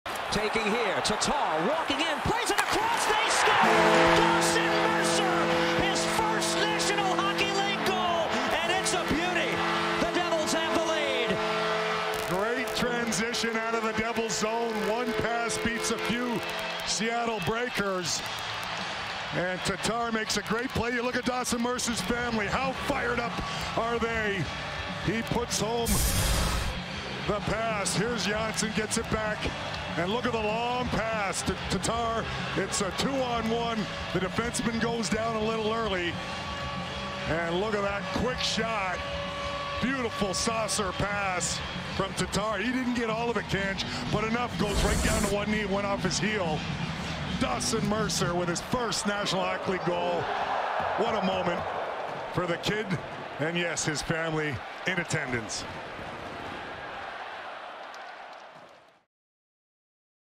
Dawson Mercer scores his first NHL goal
Dawson Mercer scored his first major league goal Tuesday night as the New Jersey Devils played the Seattle Kraken. Video: NHL/MSG+